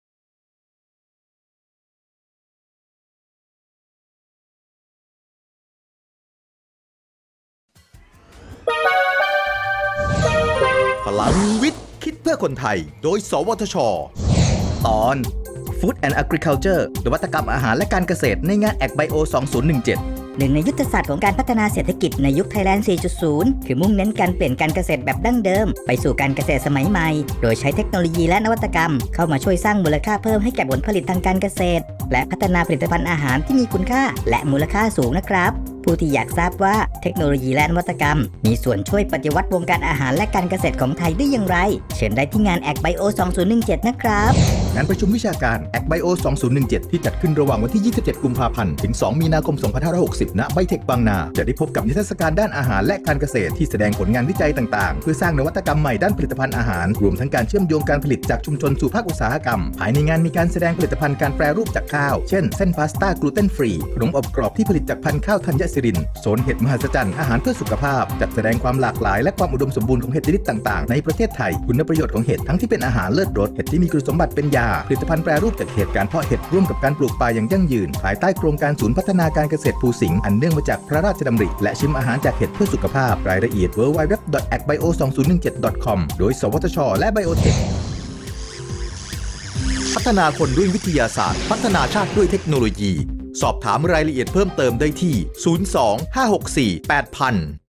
พลังวิทย์ คิดเพื่อคนไทย ตอน Food & Agriculture นวัตกรรมอาหารและการเกษตรในงาน Agbio2017
หนึ่งในยุทธศาสตร์ของการพัฒนาเศรษฐกิจในยุคไทยแลนด์ 4.0 คือมุ่งเน้นการเปลี่ยนการเกษตรแบบดั้งเดิมไปสู่การเกษตรสมัยใหม่ โดยใช้เทคโนโลยีและนวัตกรรมเข้ามาช่วยสร้างมูลค่าเพิ่มให้แก่ผลผลิตทางการเกษตร และพัฒนาผลิตภัณฑ์อาหารที่มีคุณค่าและมูลค่าสูง ผู้ที่อยากทราบว่าเทคโนโลยีและนวัตกรรมมีส่วนช่วยปฏิวัติวงการอาหารและการเกษตรของไทยได้อย่างไร เชิญได้ที่งาน AgBio 2017

งานประชุมวิชาการ Agbio 2017 ที่จัดขึ้นระหว่างวันที่ 27 กุมภาพันธ์ – 2 มีนาคม นี้ที่ ไบเทค จะได้พบกับนิทรรศการด้านอาหารและการเกษตร ที่แสดงผลงานวิจัยต่างๆ เพื่อสร้างนวัตกรรมใหม่ด้านผลิตภัณฑ์อาหาร รวมทั้งการเชื่อมโยงการผลิตจากชุมชนสู่ภาคอุตสาหกรรม

ภายในงานมีการแสดงผลิตภัณฑ์การแปรรูปจากข้าว เช่น เส้นพาสต้า gluten free ขนมอบกรอบที่ผลิตจากพันธุ์ข้าวธัญสิริน โซน “เห็ด” มหัศจรรย์อาหารเพื่อสุขภาพ จัดแสดงความหลากหลาย และความอุดมสมบูรณ์ของเห็ดชนิดต่างๆ ในประเทศไทย คุณประโยชน์ของเห็ดทั้งที่เป็นอาหารเลิศรส เห็ดที่มีคุณสมบัติเป็นยา

ผลิตภัณฑ์แปรรูปจากเห็ด การเพาะเห็ดร่วมกับการปลูกป่าอย่างยั่งยืน ภายใต้โครงการศูนย์พัฒนาการเกษตรภูสิงห์อันเนื่องมาจากพระราชดำริ และชิมอาหารจากเห็ดเพื่อสุขภาพ ดูรายละเอียดได้ที่